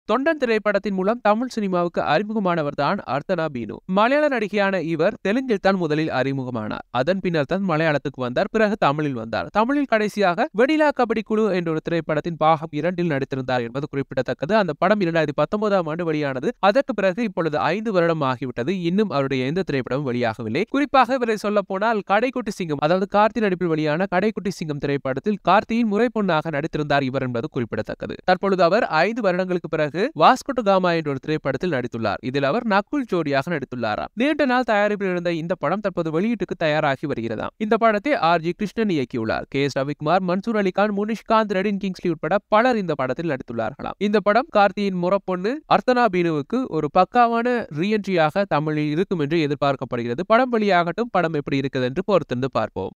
5 ஆண்டுகளுக்கு பின் தமிழுக்கு வரும் கார்த்தியின் முறைப் பொண்ணு | Karthi Movie Actress Arthana Binu
5 ஆண்டுகளுக்கு பின் தமிழுக்கு வரும் கார்த்தியின் முறைப் பொண்ணு | Karthi Movie Actress Arthana Binu